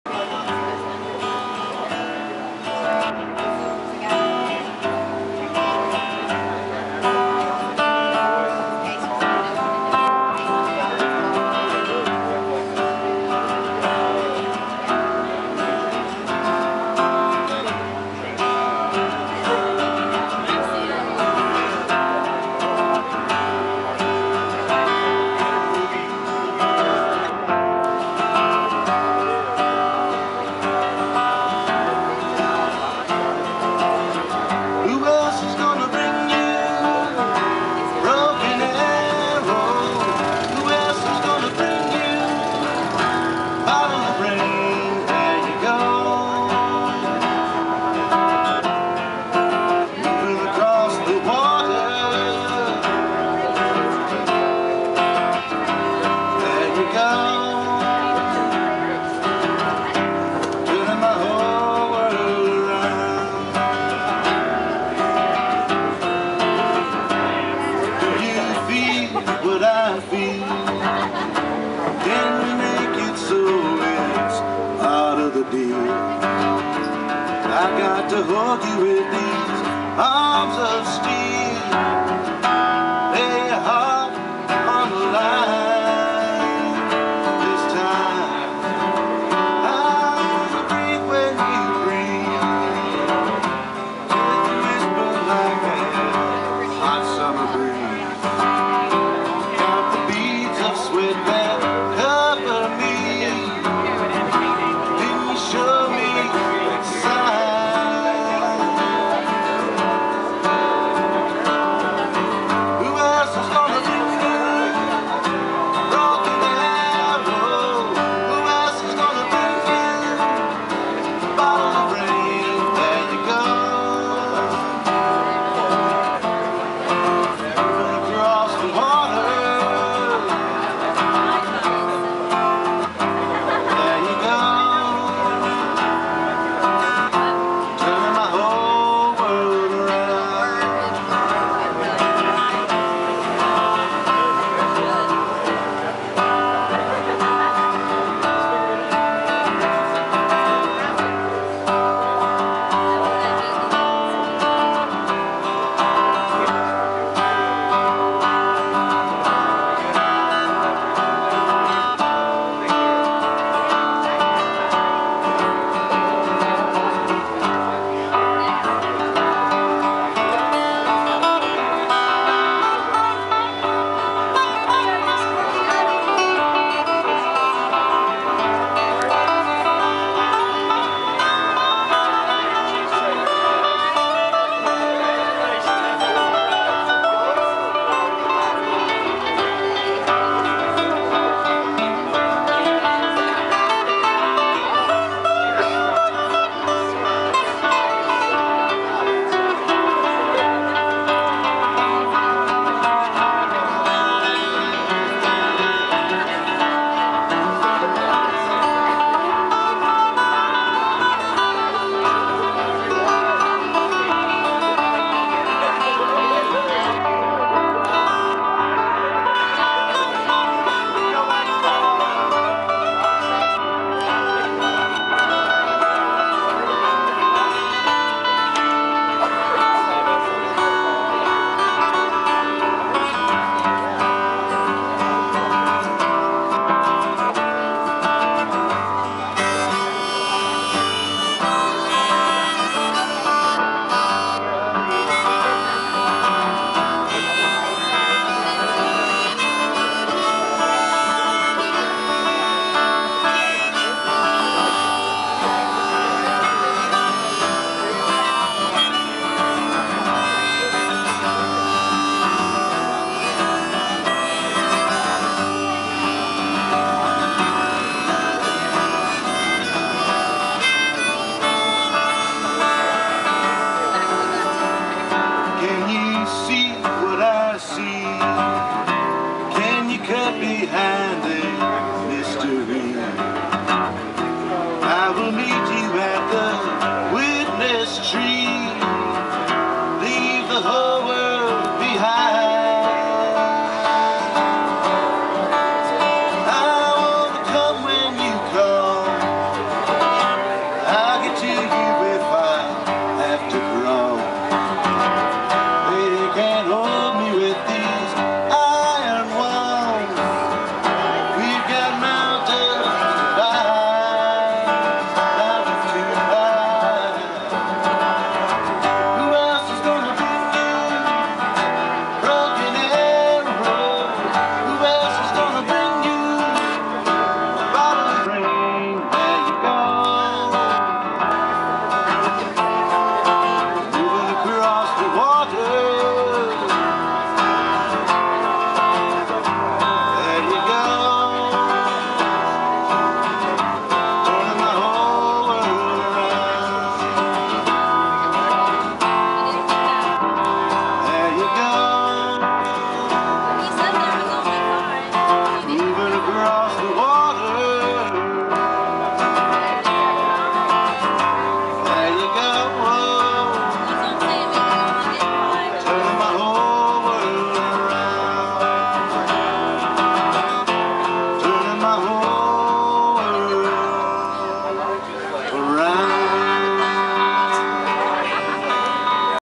Warren Dietzel DSP Video 8
Warren Dietzel performing at the Dickson Street Pub in Fayetteville, Arkansas  on Friday June 24, 2011